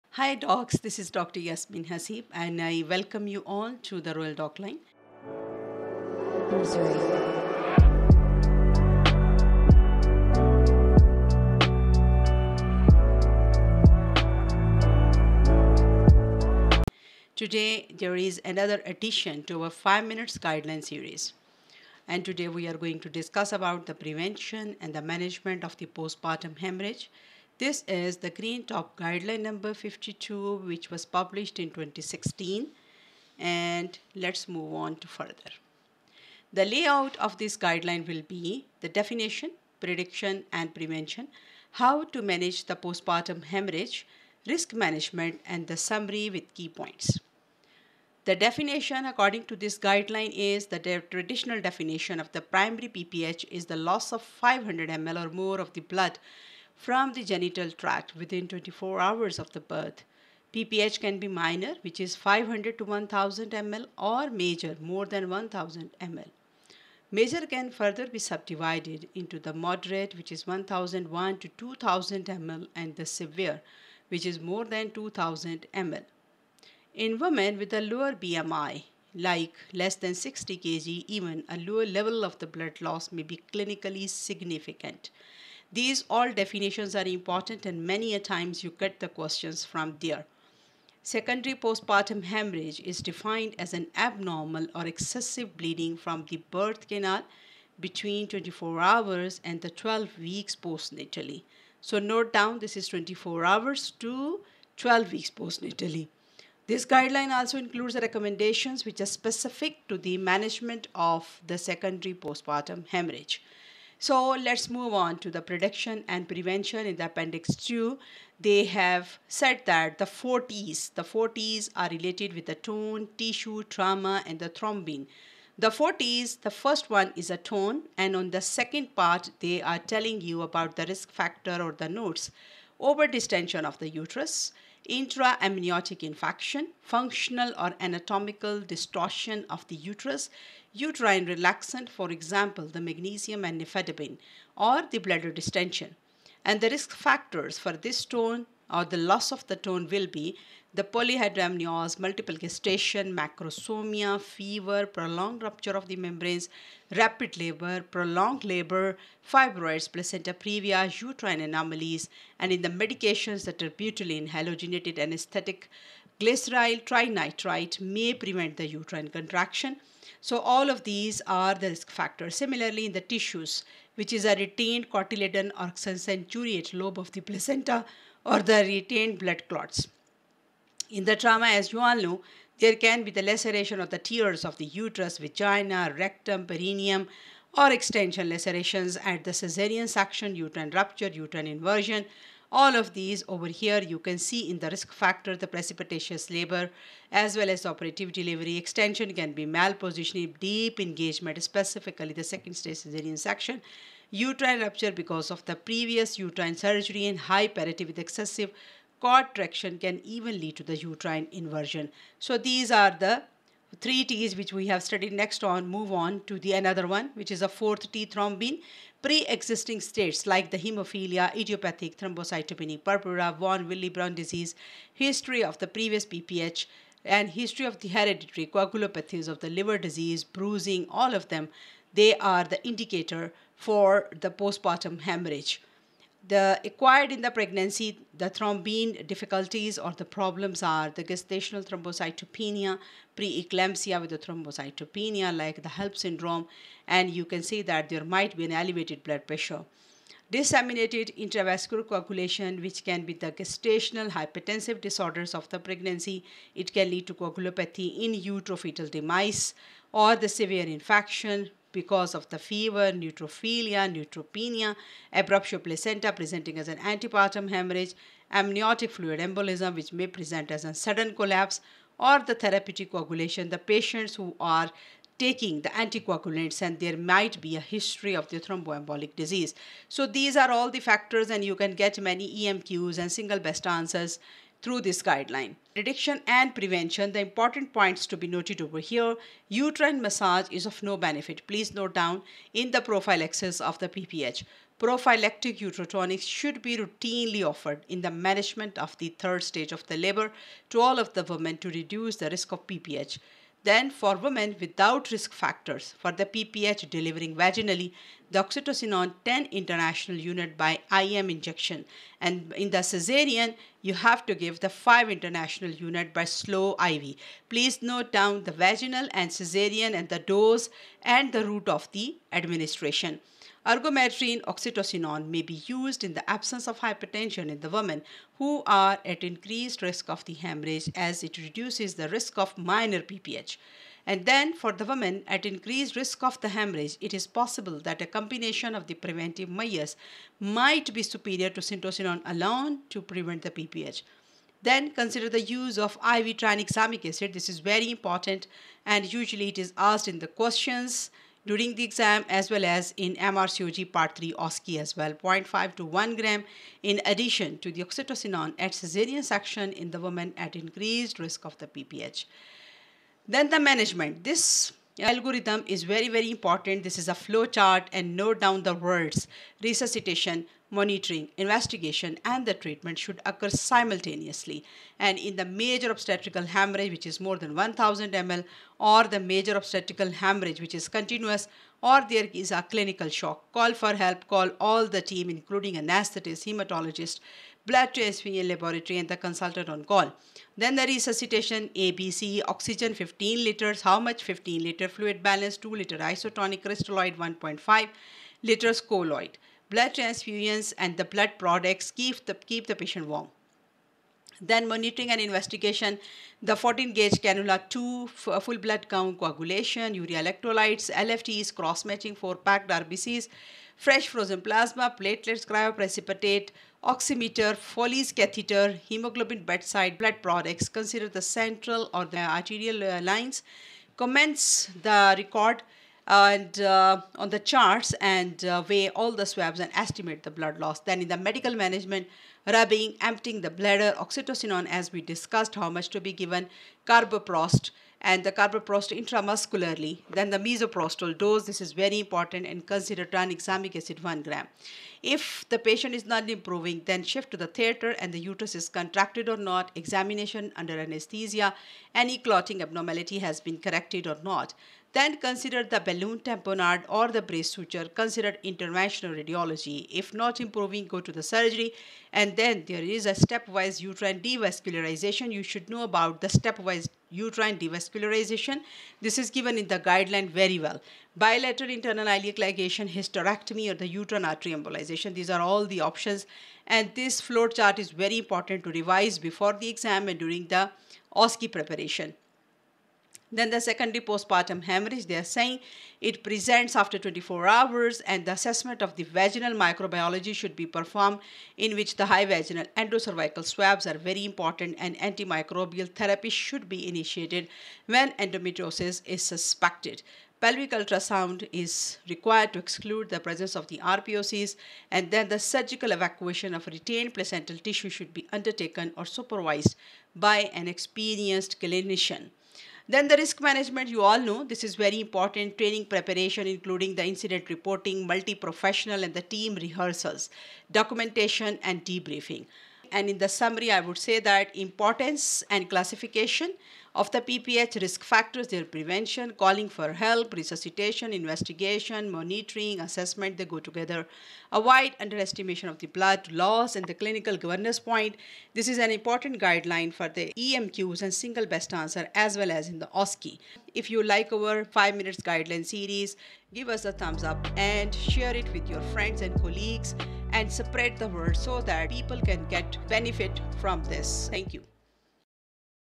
Prevention and Management of Postpartum Haemorrhage | 5-Minute Guideline | RCOG | Dr. Yasmeen Haseeb
Prevention and Management of Postpartum Haemorrhage
Green-top Guideline No. 52
Published in December 2016

If you have any questions, kindly leave them in the comments section and I’ll respond to them. Good luck with the exam!